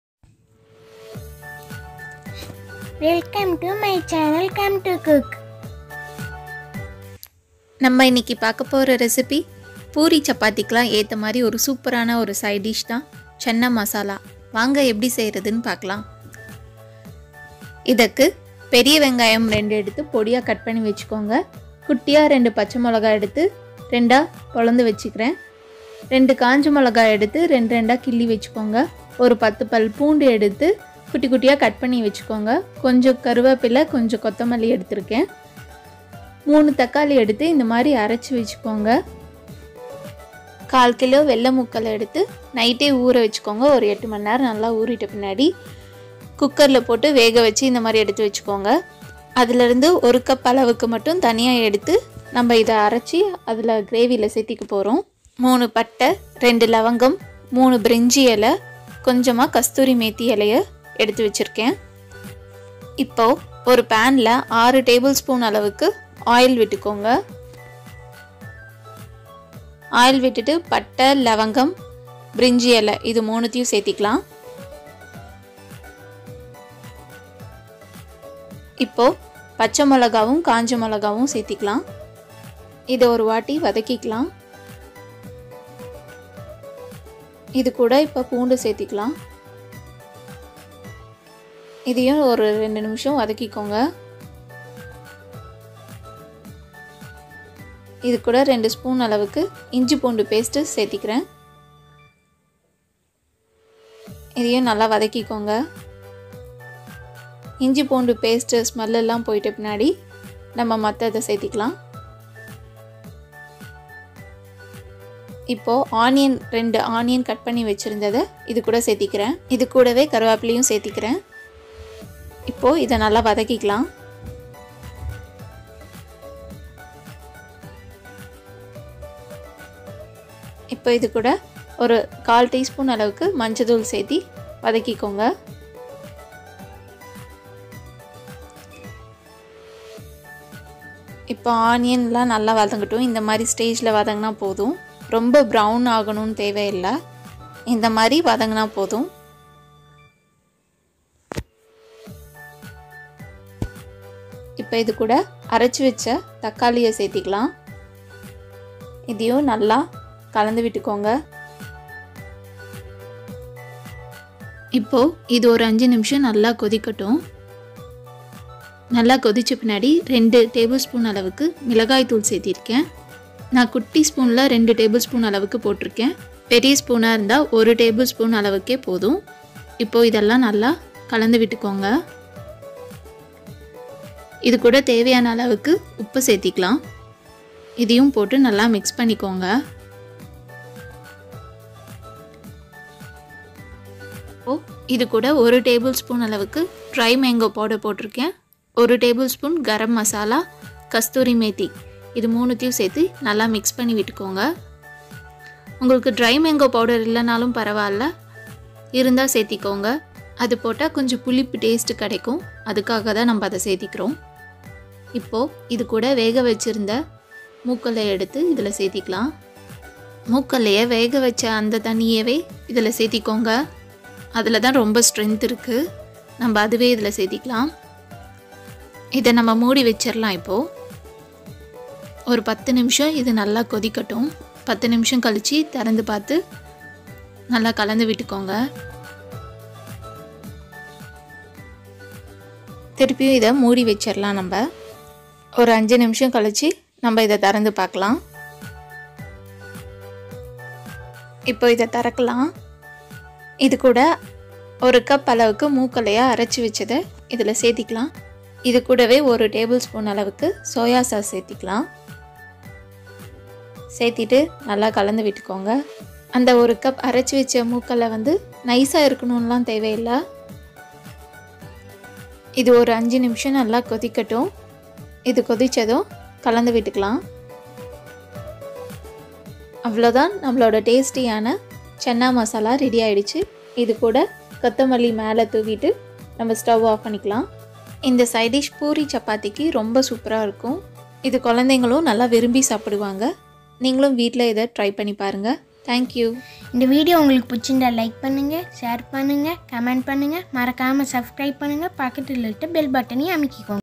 Channa masala gravy in tamil || சென்னா மசாலா கிரேவி || How to make channa masala recipe in tamil..
How to make channamasalaa gravy recipe in tamil..Super side dish for poori and chapathi.. The chick peas which is good for health and it has much of proteins.